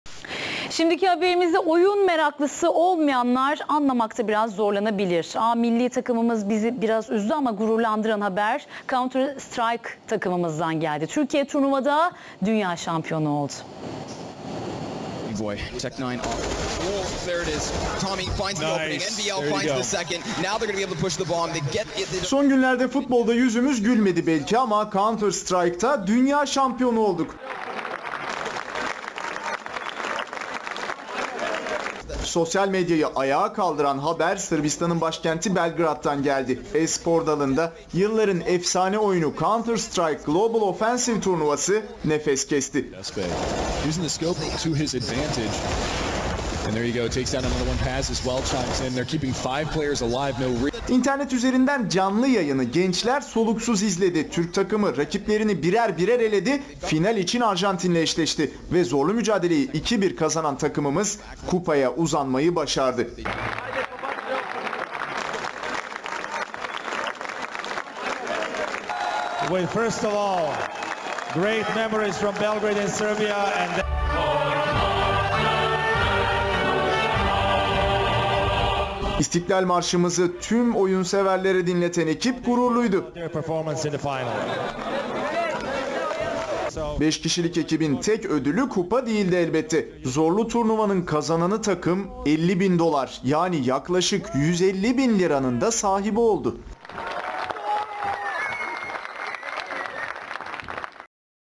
ATV CS:GO Türkiye vs Arjantin Haberi - HD
Ocak Ayında bir turnuva daha var Ödül Tam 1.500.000(bir buçuk milyon) USD - CANLI YAYINI Yine bu kanalımdan izleyebileceksiniz.
Counter Strike: Global Offensive Türk milli takımının dünya şampiyonu olduğunu ATV haber yaptı. Her ne kadar GTA IV hilelerinde biraz alay konusu olsalar da bu güzel hareketleri de çok hoş oldu doğrusu
1. raund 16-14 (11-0 den çevirdik) Kazandık
2.raund 16-5 kaybettik
3.raund 16-8 kazandık
2-1 Dünya Şampiyonu Olduk ve 50.000 USD Kazandık..